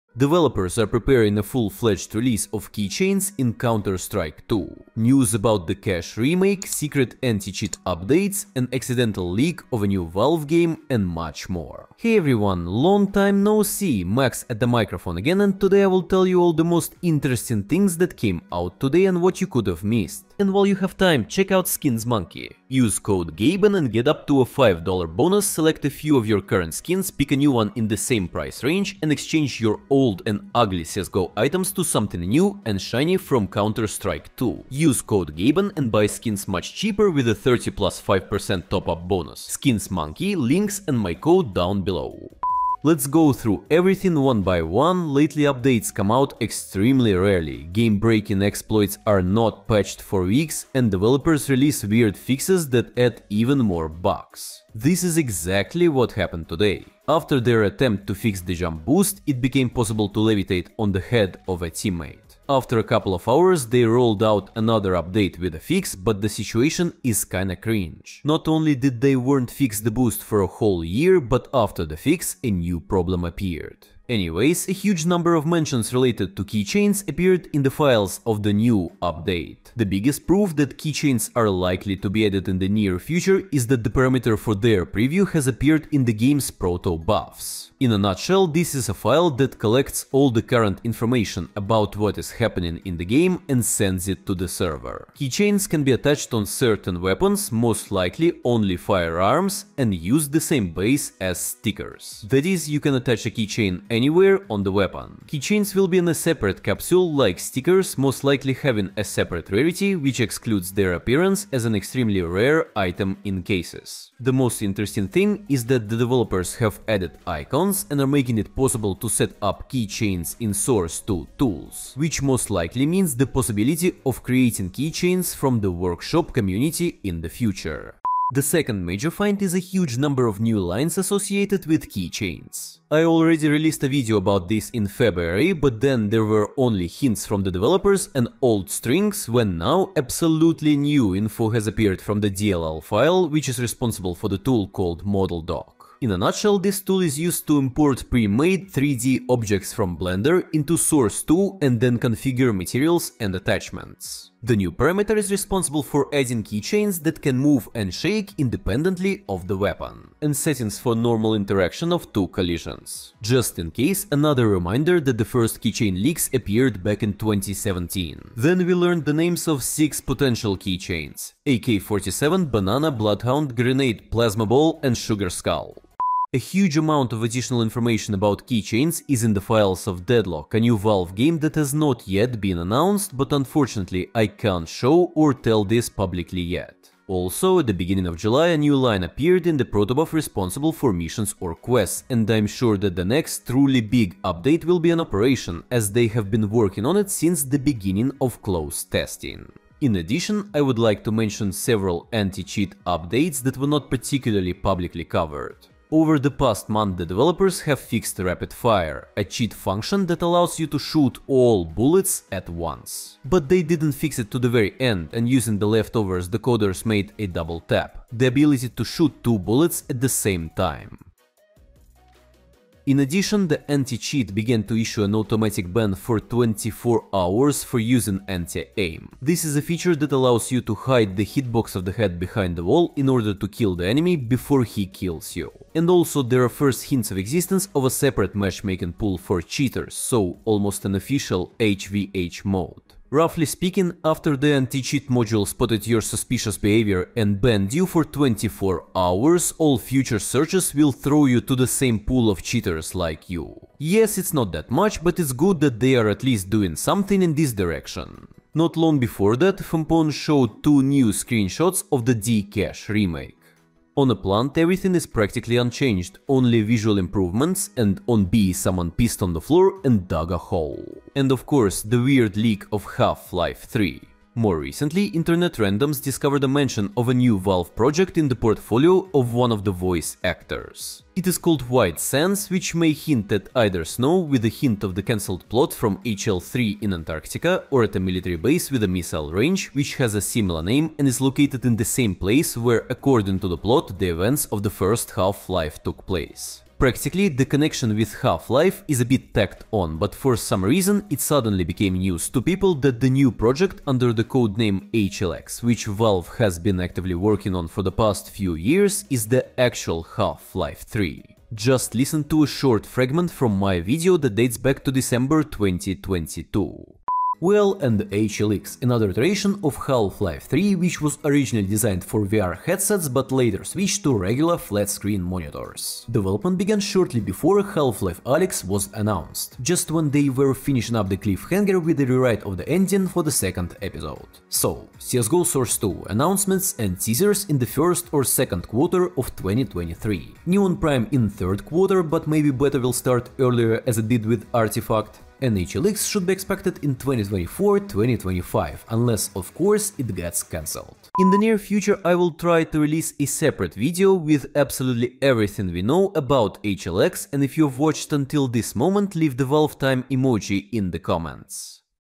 Keychains SOON in CS2 / Anti-Cheat? / HL3 Leaked - New Update
CS2 and CS:GO items / CODE: GABEN

In this video, I will talk about about the new and future update of CS2 - keychains, de_cache remake, new info on future valve game and Half-Life 3 leak, Anti-Cheat VACnet / VAC / Gabe Follower

00:00 - CS2 Update
00:40 - Devs adding bugs again
01:13 - Evidence of keychains
03:06 - First operation in Counter-Strike 2
03:19 - Anticheat improvements
04:18 - Cache remake
04:30 - Half Life 3 leak